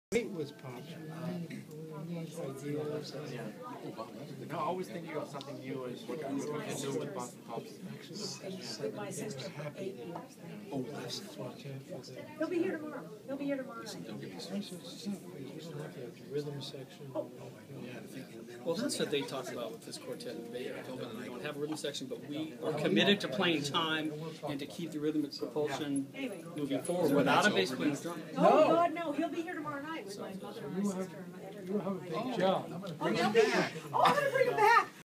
John Williams, Chris Walden & Gordon Goodwin backstage at the Hollywood Bowl.
John Williams, doing a nice job of appearing interested in what Chris Walden and I are talking about! A genius and a gentleman to boot.